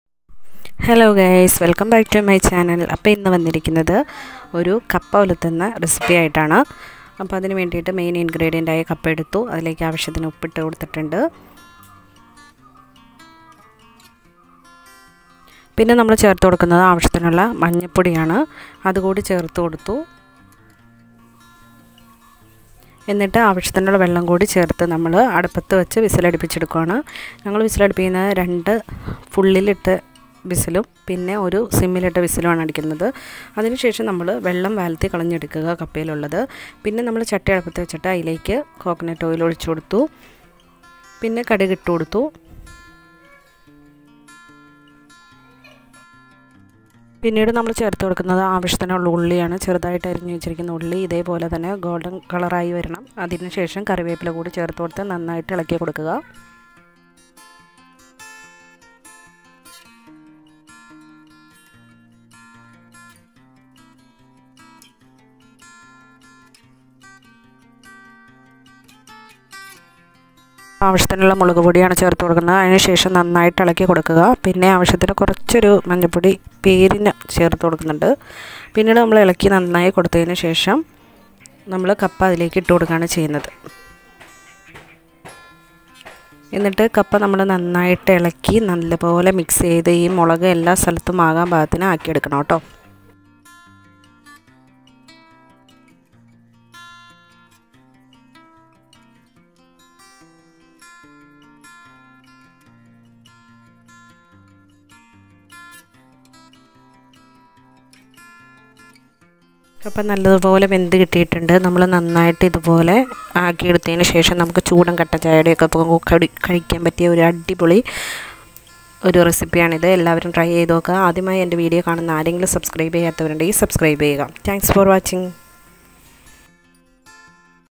kerala style kappa ularthiyath/കപ്പ ഉലർത്തിയത്
kerala dishes has been kappa. kappa ularthiyath is very simple way to make.

Ingredients :-

Tapioca
Turmeric powder
salt
chilli powder
curry leaves
 water